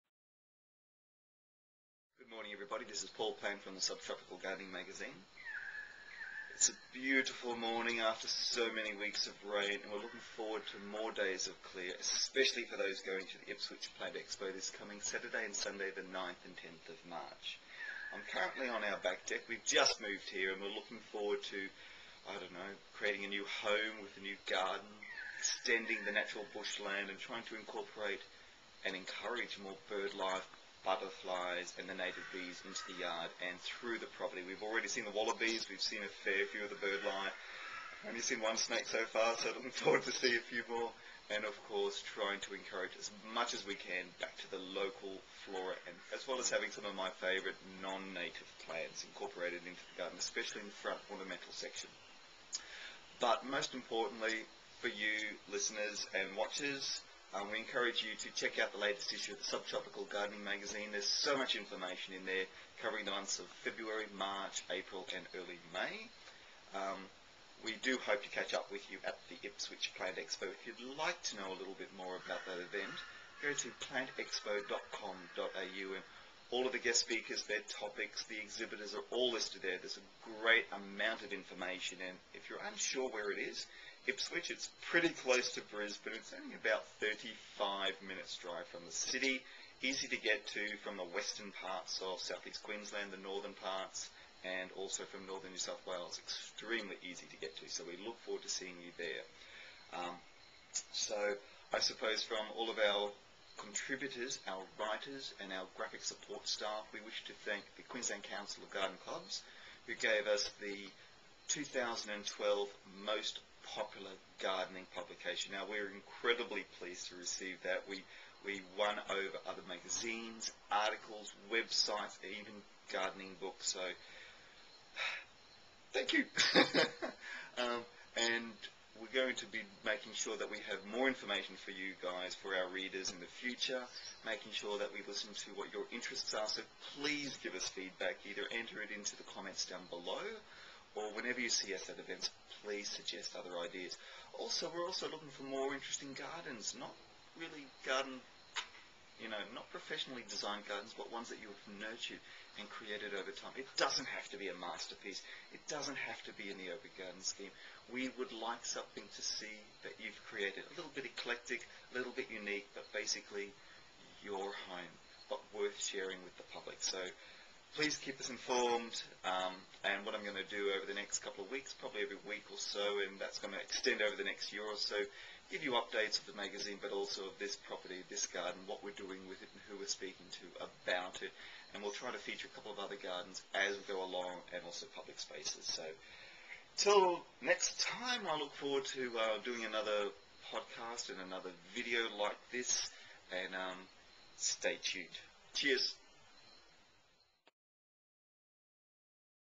subTropical Gardening with Paul Plant - 6
Paul Plant, one of Australia's leading gardening horticulturists, introduces his new home and informs viewers of the new podcasts that will begin. Don't forget to attend the Ipswich Plant Expo 9-10th March